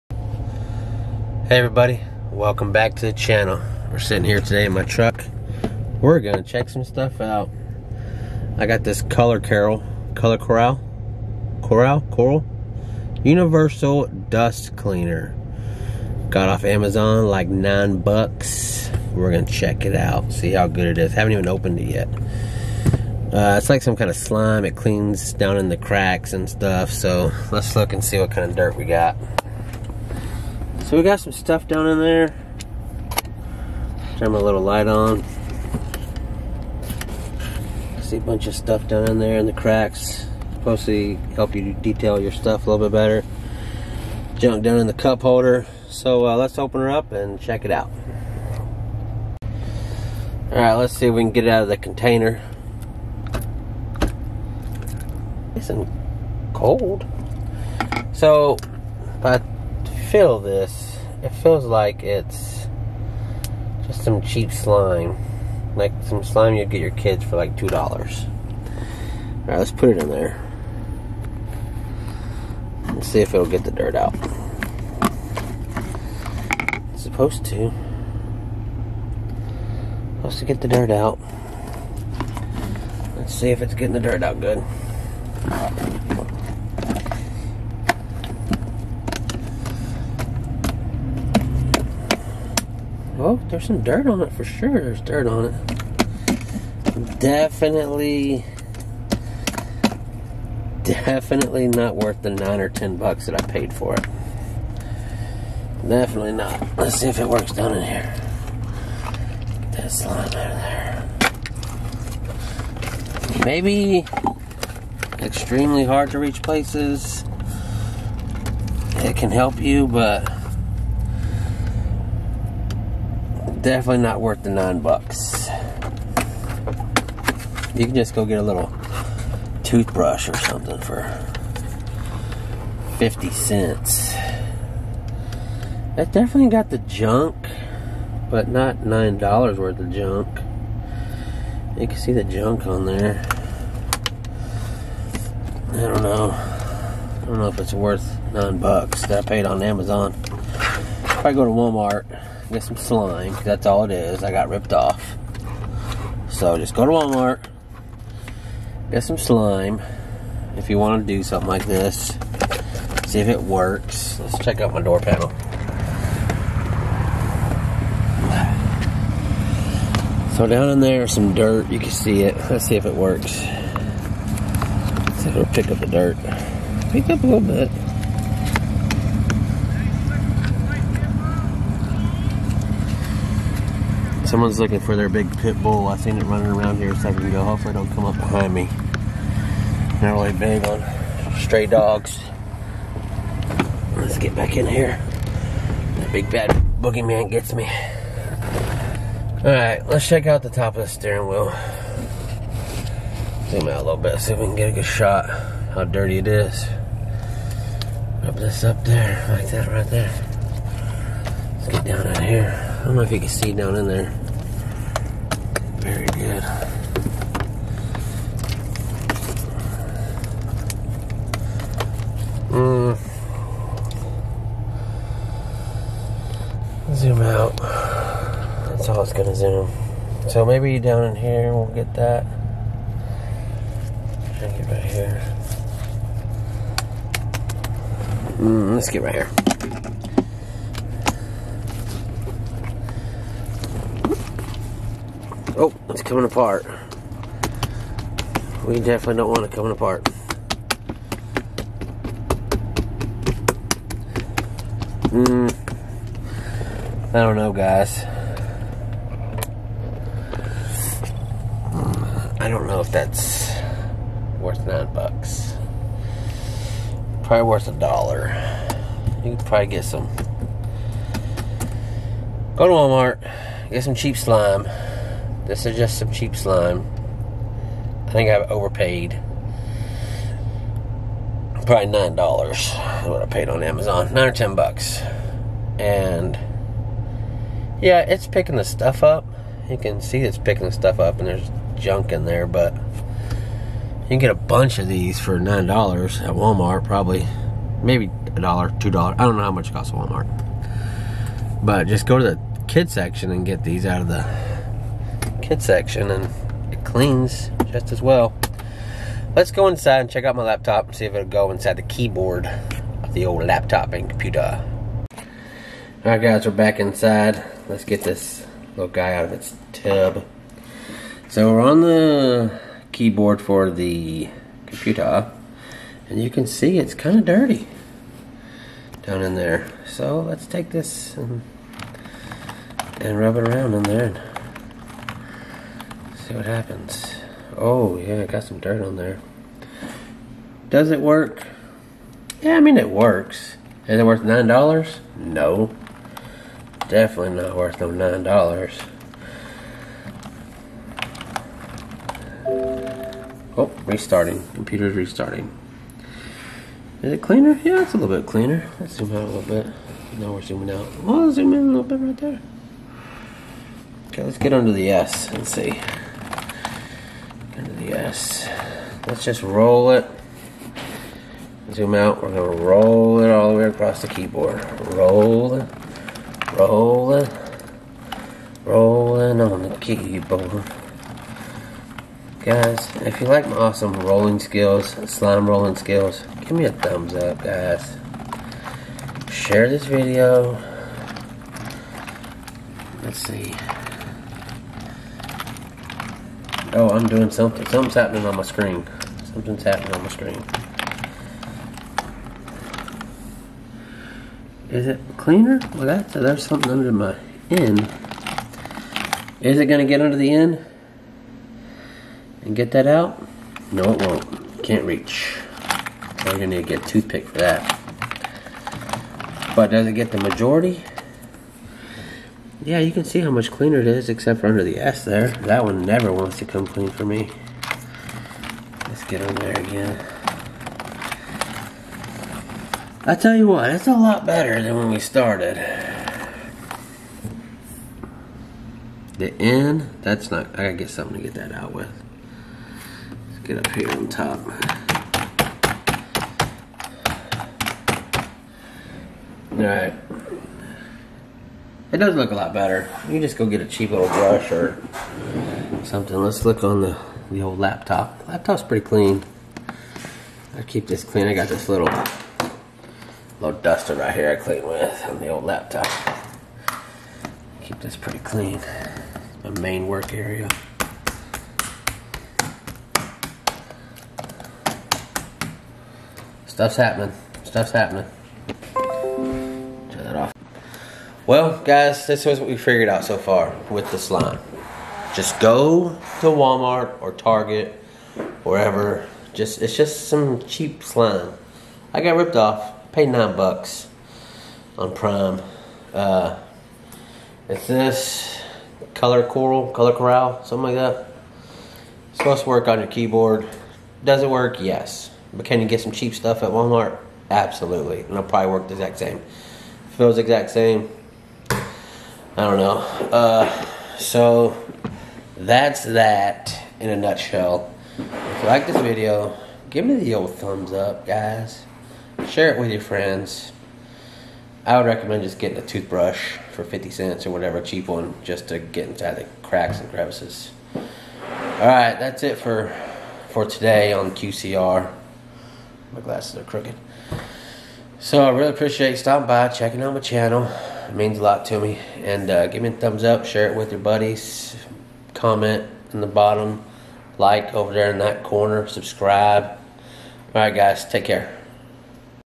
SLIME!!! Does this slime clean? NOT worth the MoNeY.
Slime. Cleaning my keyboard and inside my truck to see if this slime is worth the 9 bucks i paid for it. Please give a big thumbs up. Thank You for stopping by my channel.